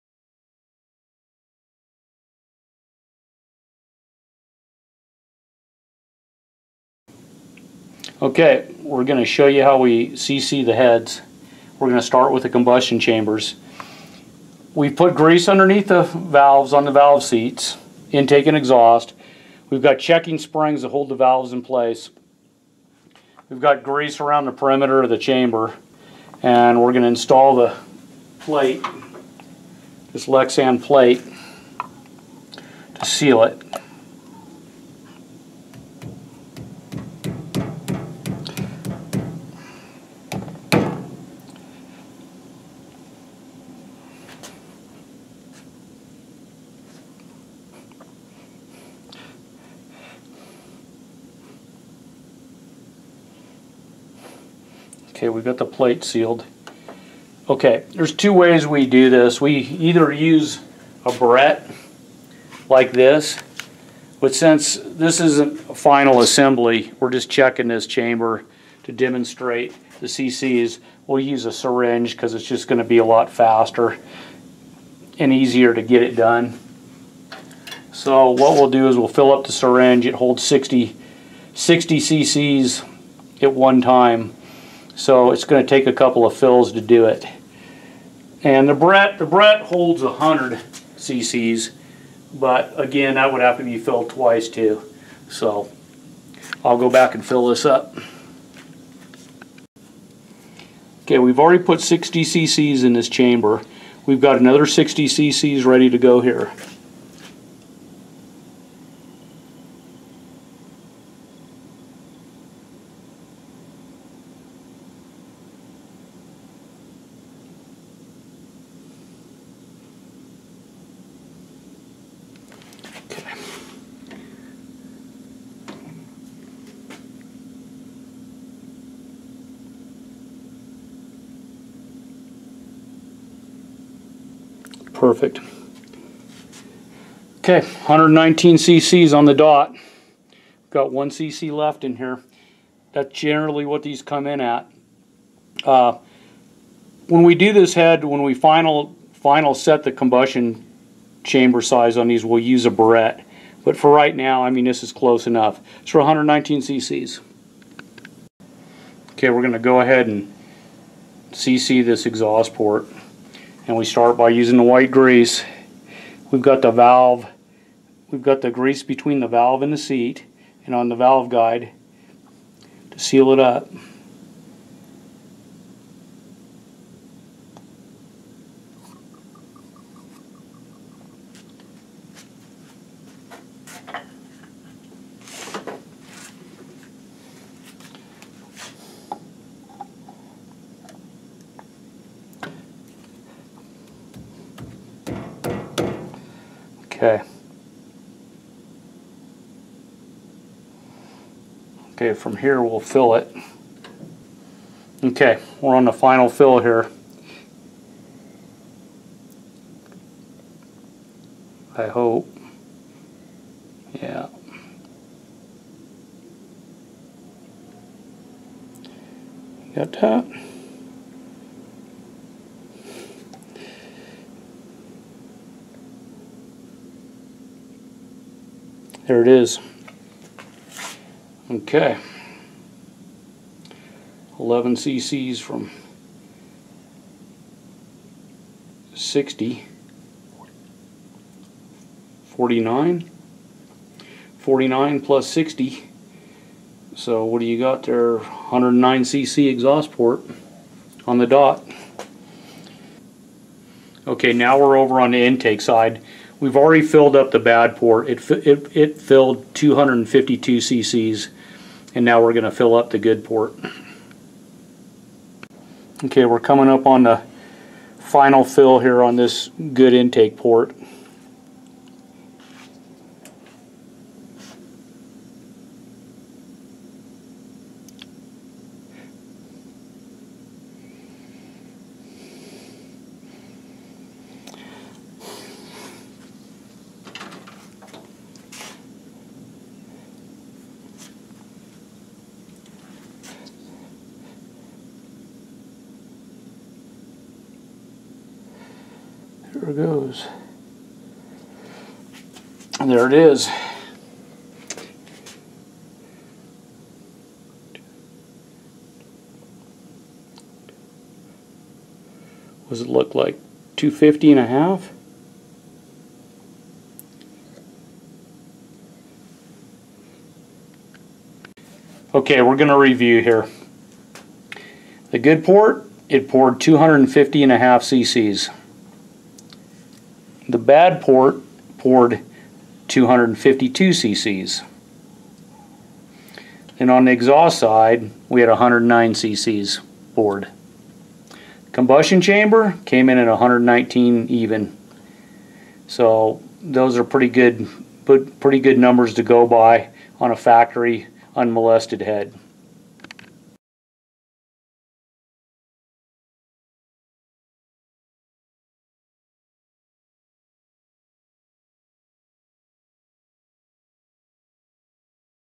Checking GM 781 Cylinder Head Chamber and Port Volume Street/Strip 454 Big Block Chevy
Part 13 - Checking GM 781 Cylinder Head Chamber and Port Volume
-In Depth Performance Engine Build
620+ Horse Power Street/Strip Pump Gas 454 Big Block Chevy